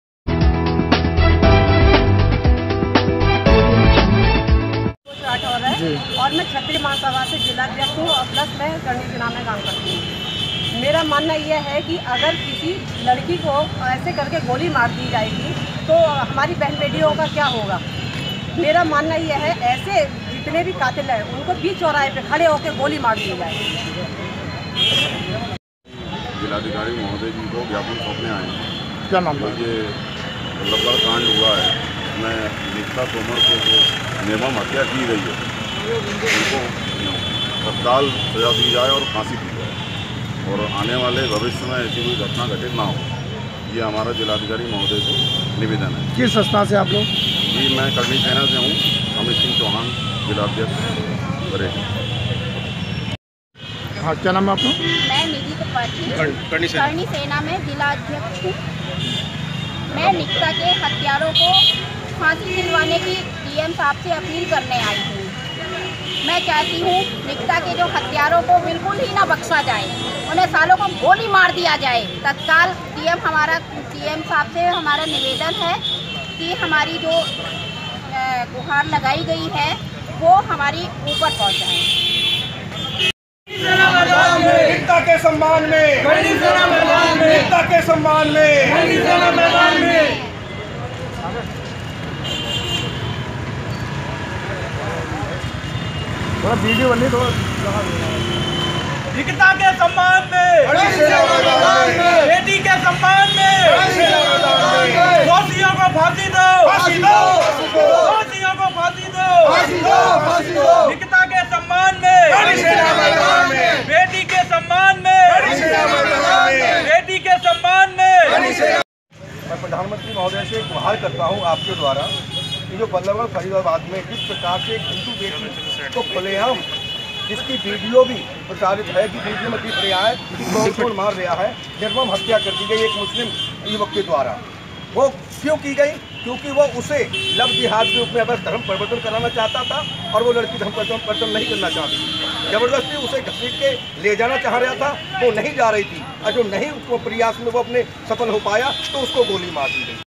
बरेली न्यूज़: करणी सेना ने किया धरना प्रदर्शन जिलाधिकारी के माध्यम से प्रधानमंत्री को दिया ज्ञापन
बरेली आसपास के समाचार
बरेली न्यूज़ जिला मुख्यालय पर करणी सेना ने जोरदार प्रदर्शन किया जिलाधिकारी के माध्यम से माननीय प्रधानमंत्री भारत सरकार नई दिल्ली को ज्ञापन भेजा मांग की हरियाणा में बल्लभगढ़ फरीदाबाद मैं जिस प्रकार से एक हिंदू की बेटी को खुलेआम निर्भयता से एक मुस्लिम युवक द्वारा हत्या कर दी गई वह हमारी लचर कानून व्यवस्था आने वाली भयंकर विपत्ति का धोखा है यदि इस लव जिहाद के चलन के विरुद्ध उचित कानून व्यवस्था नहीं बनाई गई तो वह दिन दूर नहीं जब हिंदुओं को अपनी सुरक्षा की व्यवस्था स्वयं करनी होगी हमारी मां बहनों के सम्मान जीवन की रक्षा सरकार का दायित्व है उसके लिए उचित कानून अति शीघ्र बनाना जाना चाहिए